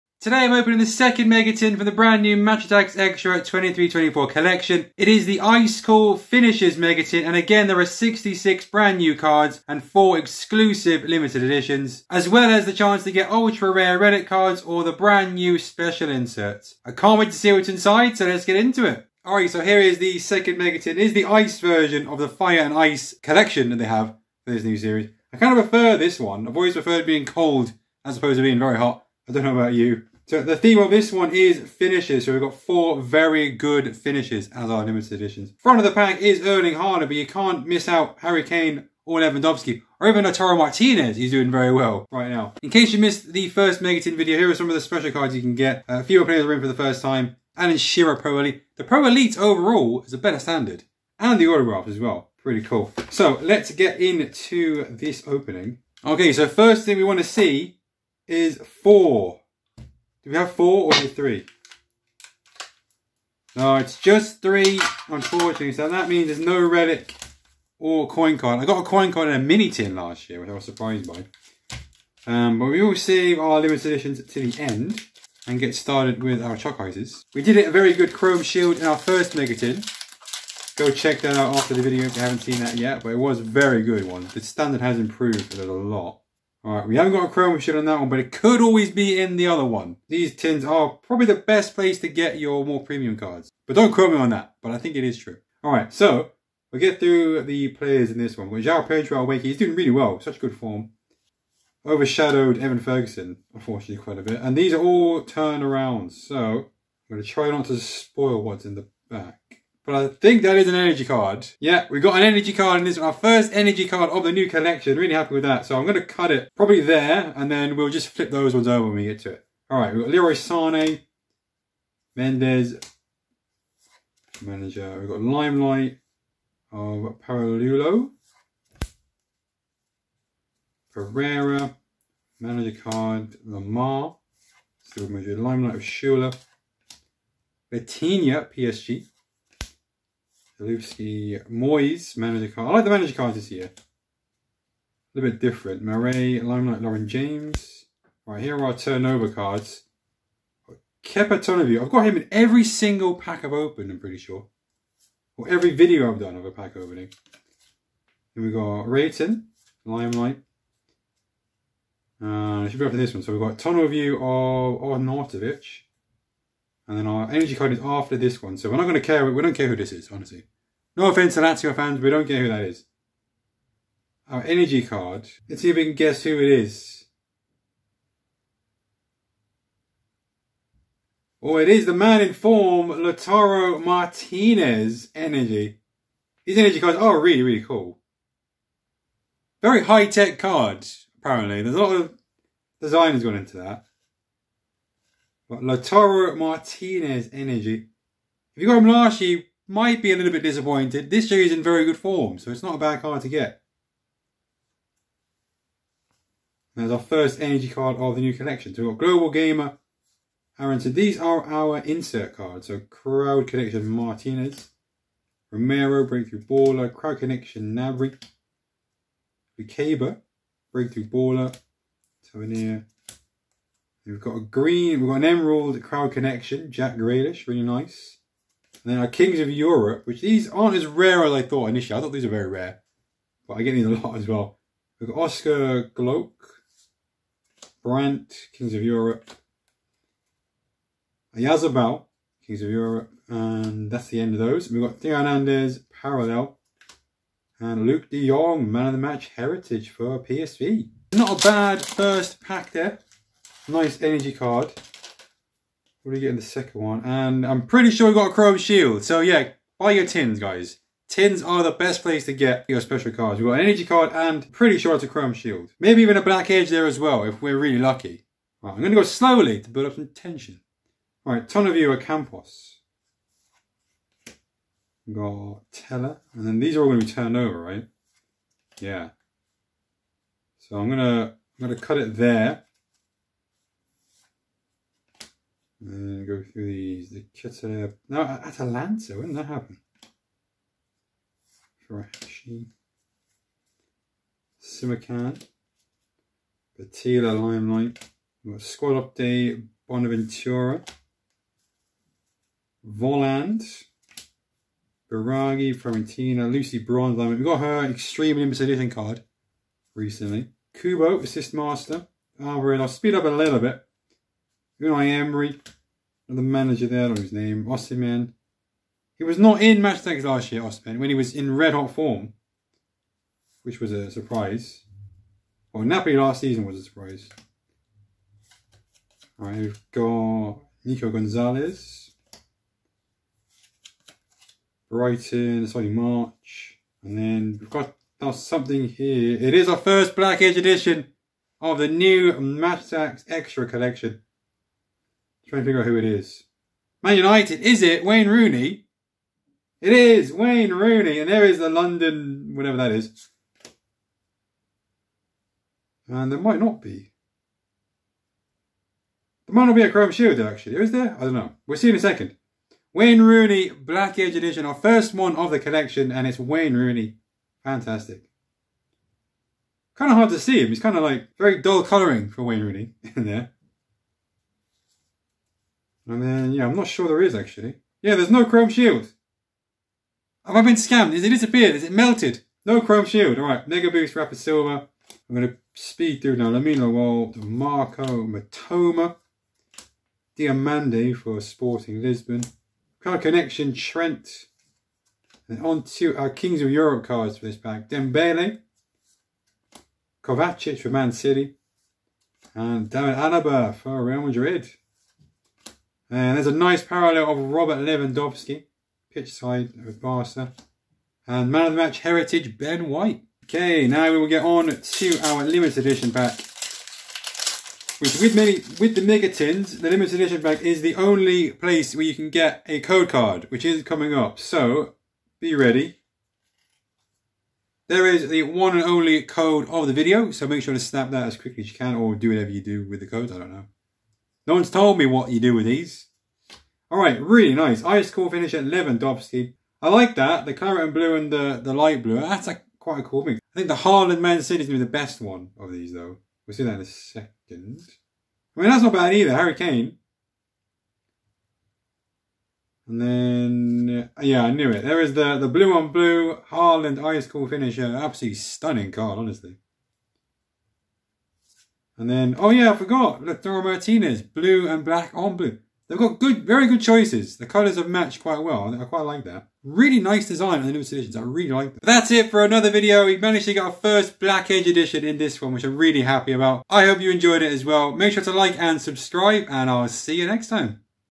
MY FIRST BLACK EDGE AND ENERGY CARDS!! SECOND MEGA TIN! MATCH ATTAX EXTRA 23/24!
Join me as I open the second mega tin from the BRAND NEW MATCH ATTAX EXTRA 23/24 collection from TOPPS! This one is called ICE COOL FINISHERS and contains 66 brand new cards and 4 exclusive LIMITED EDITIONS!

Match Attax Extra 2024 is an action-packed update collection of 344 cards full of unique designs and categories bringing you closer to the game and your favourite footballers. There are more Black Edge cards, more Energy cards, more Chrome X cards, more auto and relic cards and more Crystal and Emerald parallels to collect than ever before. Plus all new categories such as Chrome X Pro-Elite, UCL Debut relic cards, Extreme Limited Editions, a Harry Kane Dream Debut card and for the first time Match Attax Extra features the stars of the UCWL.